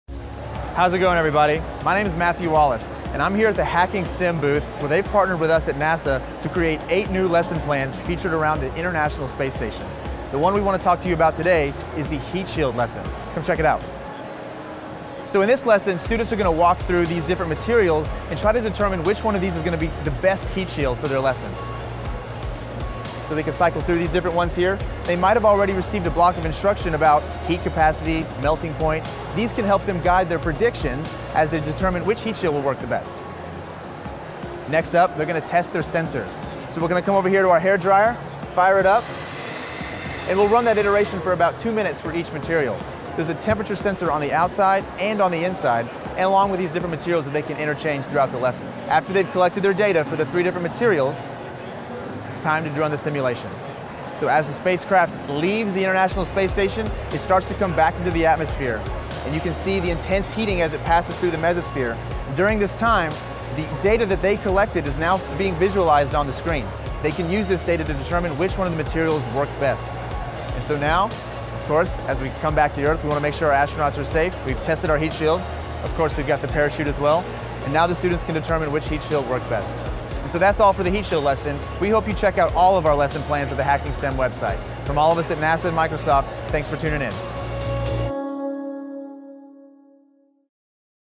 Partner Spotlight: Got STEM? NASA lesson plans hit on science, technology, engineering and math
We’ve teamed up with NASA to create eight free lesson plans for middle and high school students that are now available in our Hacking STEM library. Each space-themed STEM lesson plan emphasizes project-based learning and encourages critical thinking and creative problem-solving. Students design in 3D, analyze data, build sensors, use virtual reality and more.